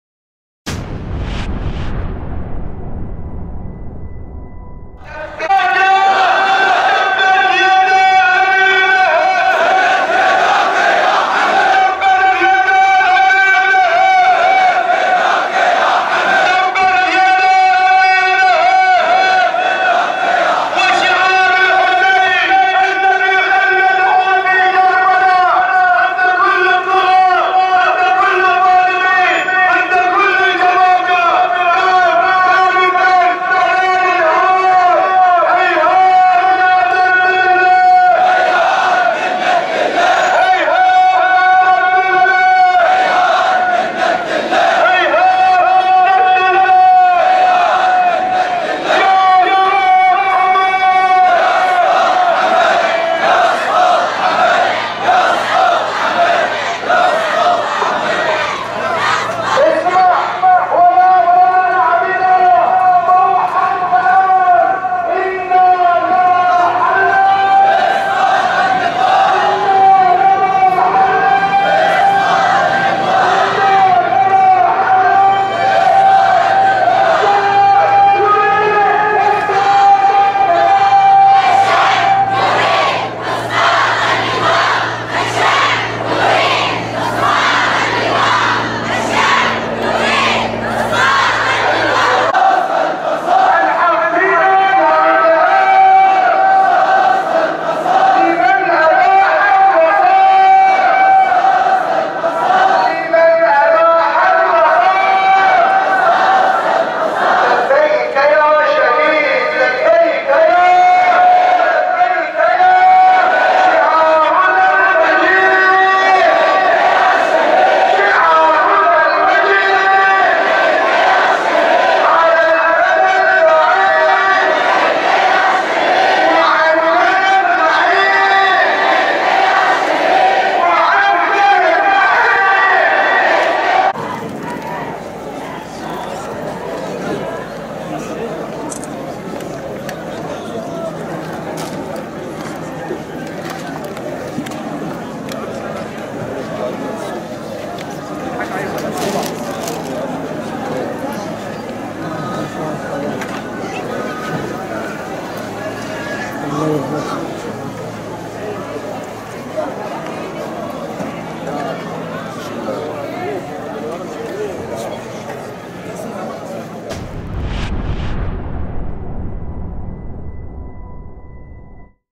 مشاهد جوية من مسيرة ختام فاتحة الشهيد احمد عبدالامير - بلدة السنابس 3-12-2013
البحرين - بلدة السنابس : مشاهد جوية من مسيرة ختام فاتحة الشهيد احمد عبدالامير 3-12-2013

T : @Feb14Media
14FebGallery
T: @14FebGallery
14Feb Media Network Eng - Fr
T: @Feb14MediaEN
BlackBerry :
Pin:25D14785